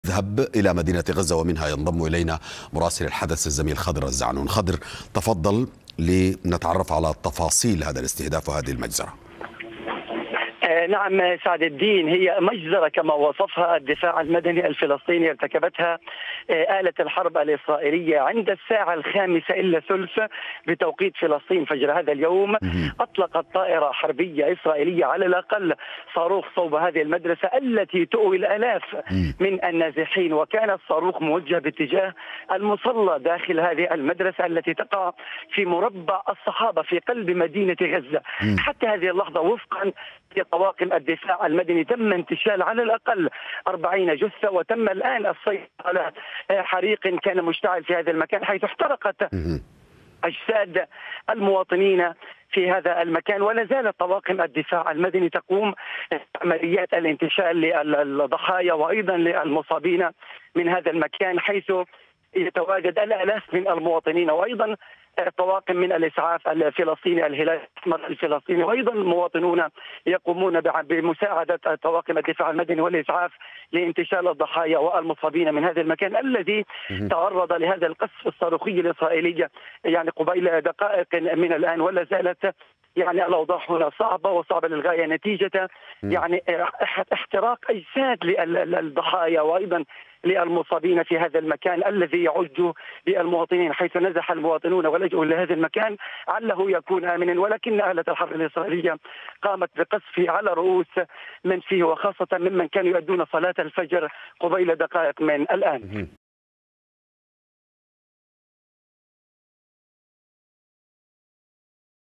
قصف إسرائيلي يستهدف مصلى داخل مدرسة التابعين في غزة
مراسل الحدث خضر الزعنون: قصف إسرائيلي عنيف استهدف مصلى في مدرسة التابعين تؤوي آلاف النازحين شرقي مدينة غزة أثناء أداء صلاة الفجر